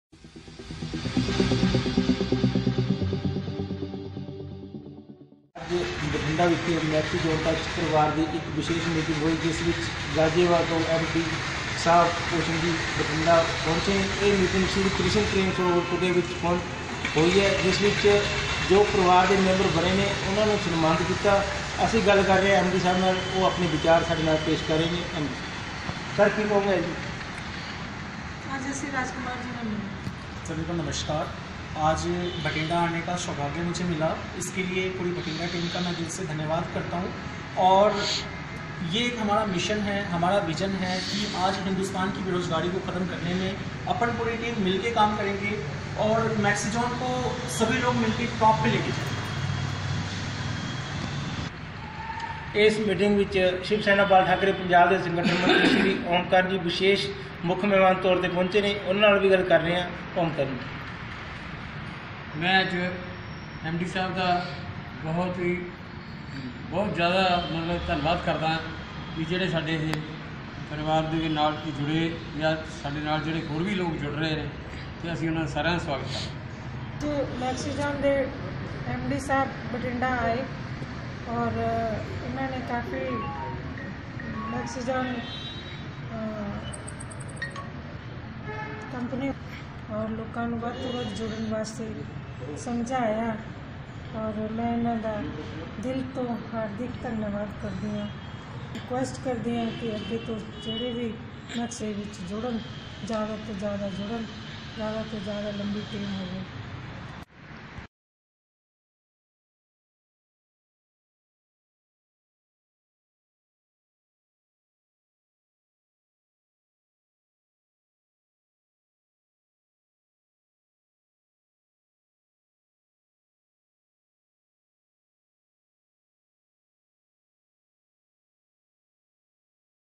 BATHINDA: MAXIZONE TOUCH PARIVAR KI VISHESH MEETING# Sh OM KARAN BHI MEETING ME SHAMIL
मैक्सी जॉन टच परिवार देश की तरक्की में अपना योगदान देगा भूषण सिंह जी
 बठिंडा से दीपक के साथ राजकुमार की रिपोर्ट

बठिंडा में आज मैक्सी जॉन टच परिवार लिमिटेड की एक विशेष मीटिंग हुई जिसमें शिवसेना बाल ठाकरे पंजाब के संगठन मंत्री श्री ओम करण जी मुख्य में मान के तौर पर पहुंचे उन्होंने कहा कि मानवता की सेवा को मुख्य रखते हुए कार्य कर रहे है बहुत ही अच्छा कार्य है इस मौके पर श्री भूषण सिंह जी पुनीत जी विशेष तौर पर पहुंचे और उन्होंने कहा कि मेक्सिजोन टच परिवार देश की तरक्की में विशेष योगदान देगा इस मौके पर राजकुमार और सभी परिवार के मेंबरों ने आए मेहमानों का स्वागत किया इस मौके पर डॉ सुखलेश रानी के इलावा विक्की छिंदा प्रदीप जी बलवीर जी और सभी टीम के मेंबरों ने आए मेहमानों का स्वागत किया और उनको सम्मानित गीत